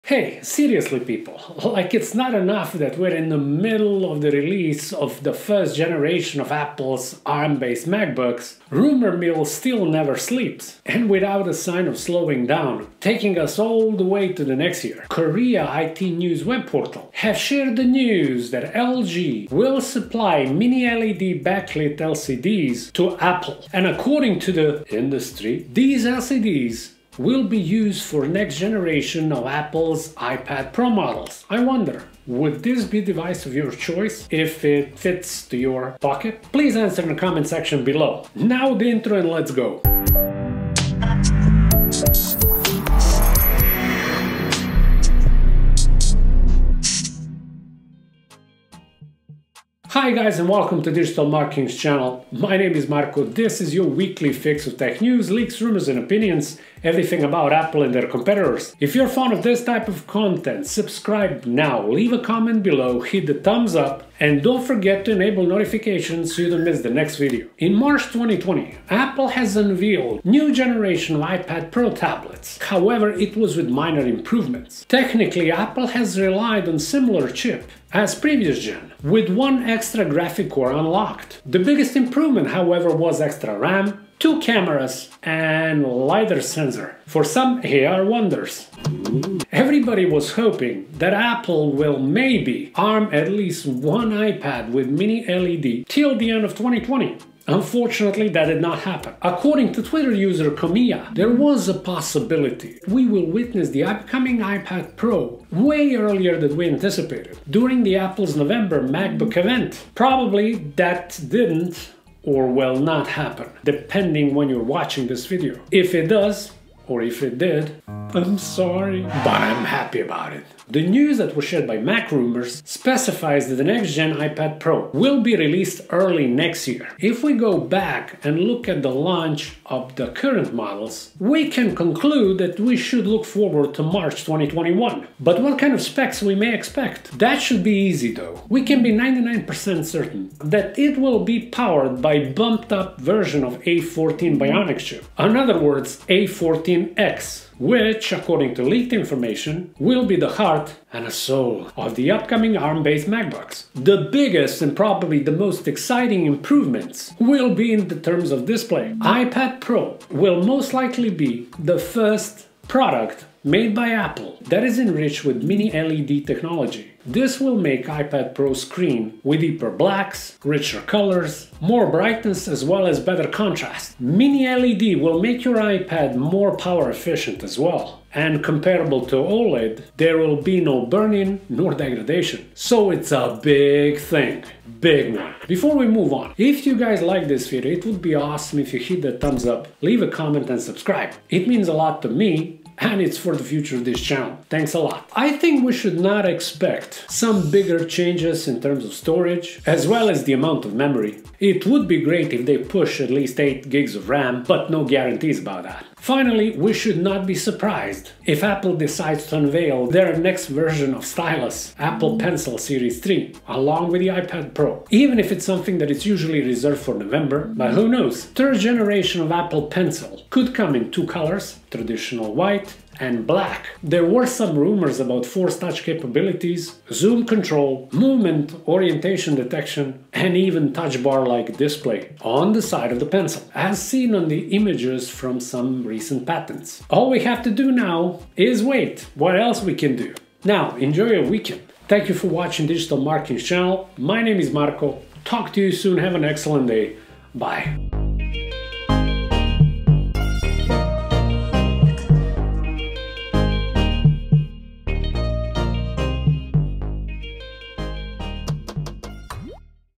Apple iPad Pro 5th Generation 2021, Mini LED, Apple pencil 3
Apple iPad Pro 5th Generation, seems like release date will be March 2021. The biggest improvement will be all new Mini LED display. But that's not all.  Check out the second part of this video, about Apple Pencil 3

What we know for certain is that the iPad Pro 5, will be powered by Apple's A14X Bionic chip. It is possible there will be no changes in terms of RAM and storage configurations, but Apple could surprise us. There are some exciting predictions about the next generation Apple Pencil 3.

0:00 Introduction
1:07 iPad Pro 2021with Mini LED
3:05 More details and Apple Pencil 3
3:57 Closing